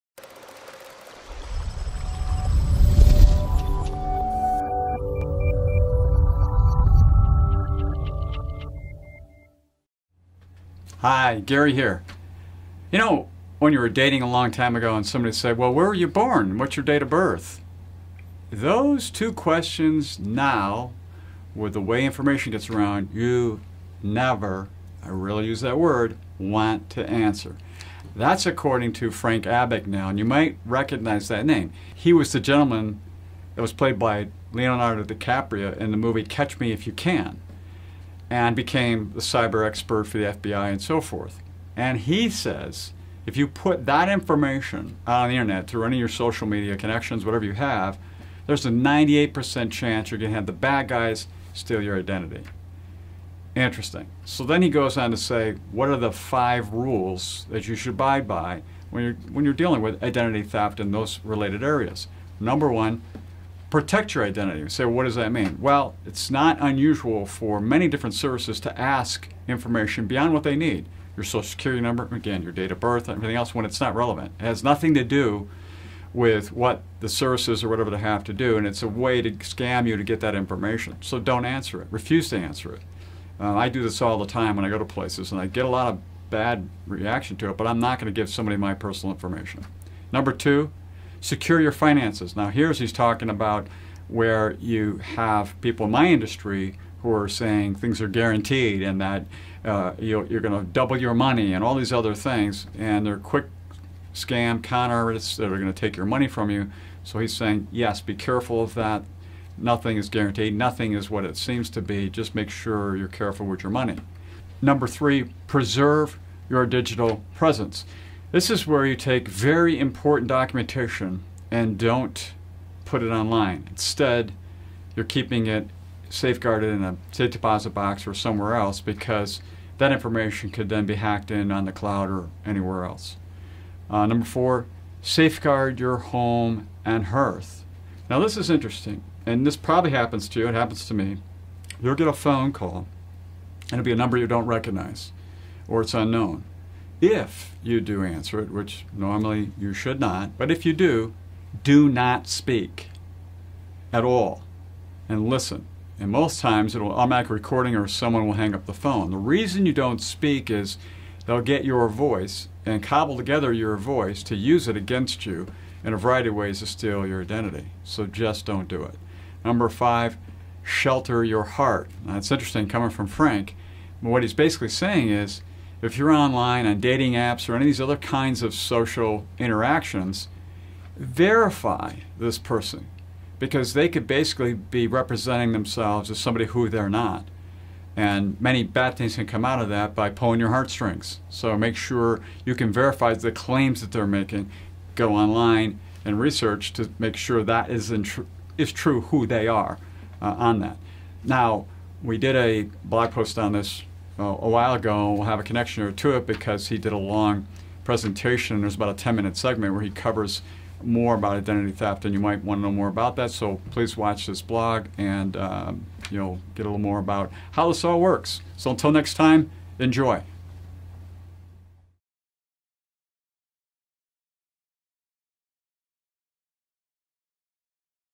Five Rules to Abide By When Dealing With Identity Theft
Never readily give away your personal information. By putting your personal information on social media, you have a very high probability of having your identity stolen. There are rules you can follow to help protect yourself from identity theft. To learn more about this blog post follow this